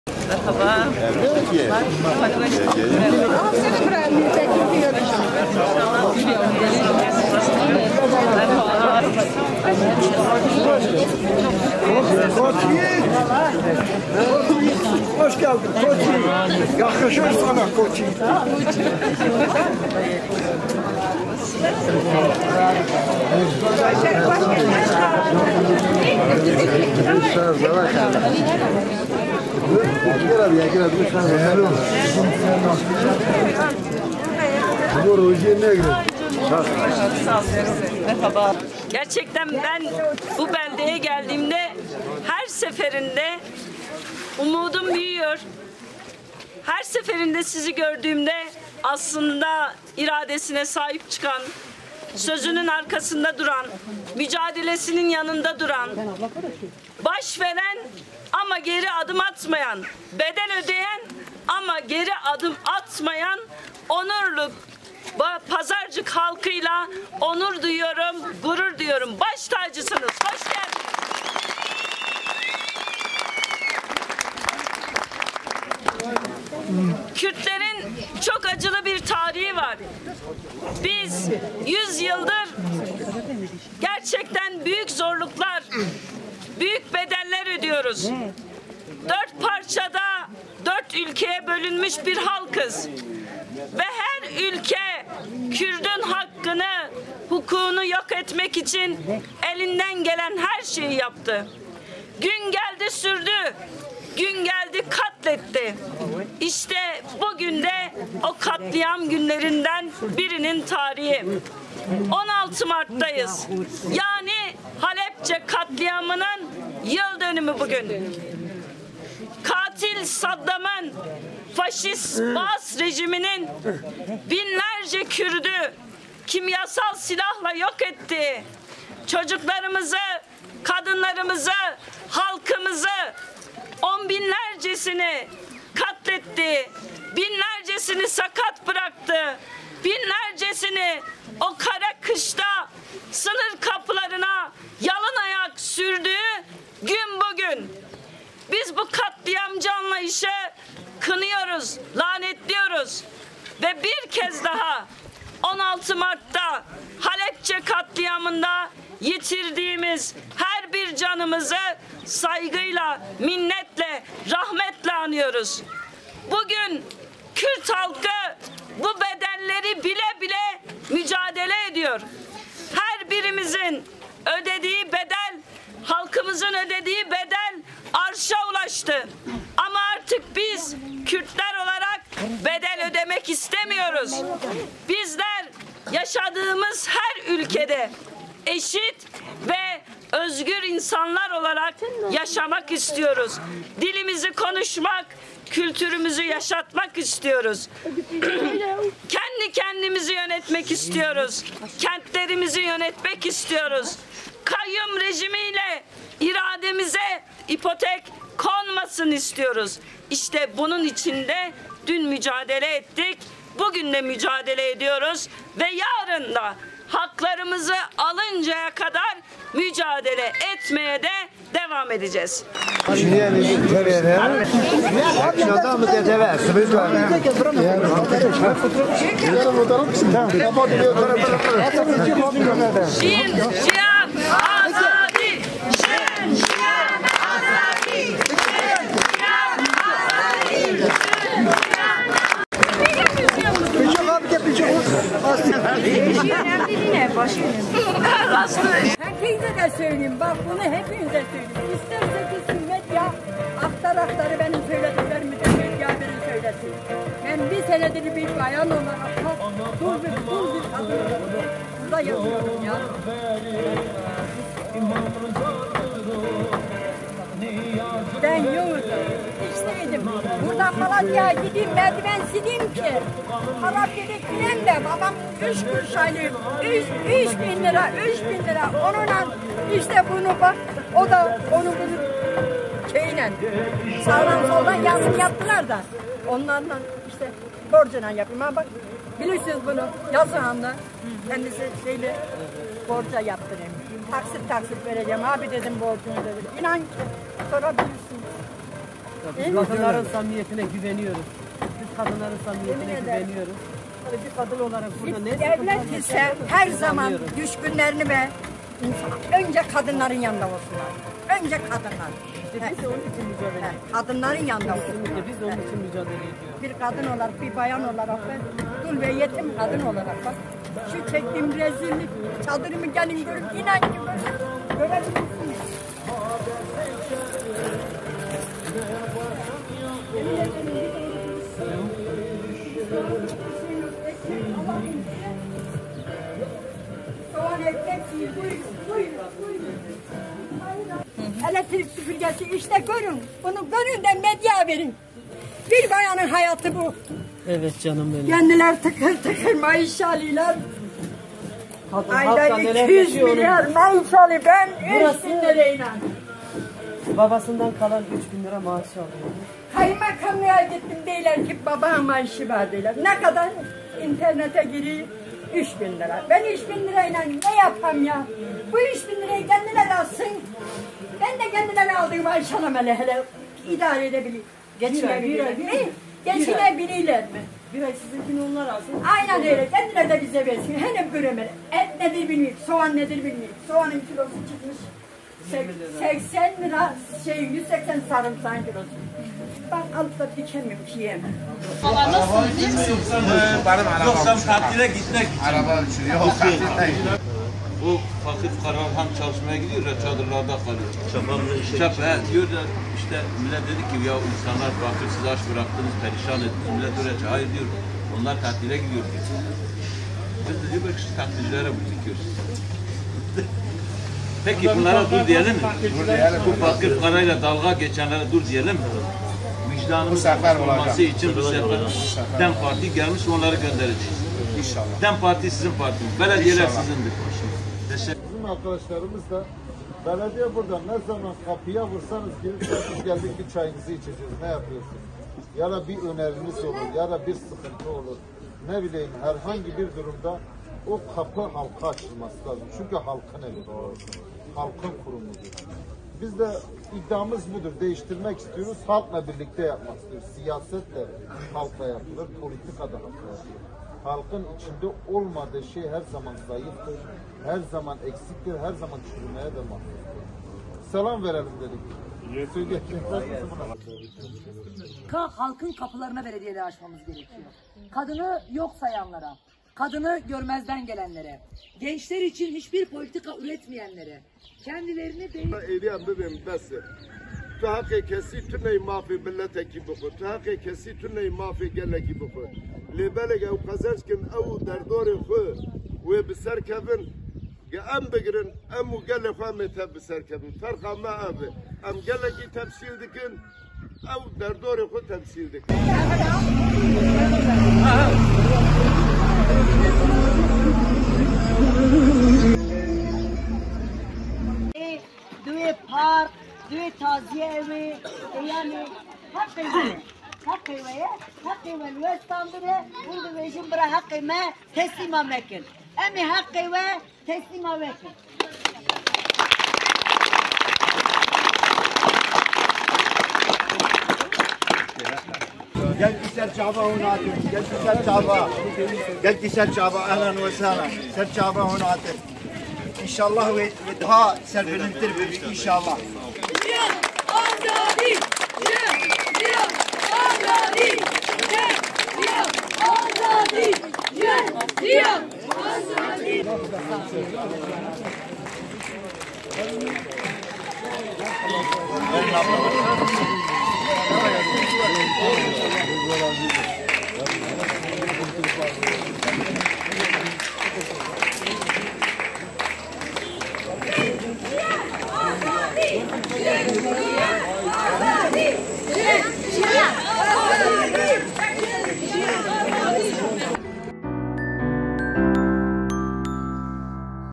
DEM ADAYLARI SOKAKTA, HALK ÇARE İSTİYOR
DEM Parti eşbaşkan adayları ve partililer, kent kent, sokak sokak yürüttüğü çalışmalarda projelerini anlatırken, halk yaşadığı sıkıntılara karşı çare bulmalarını istedi.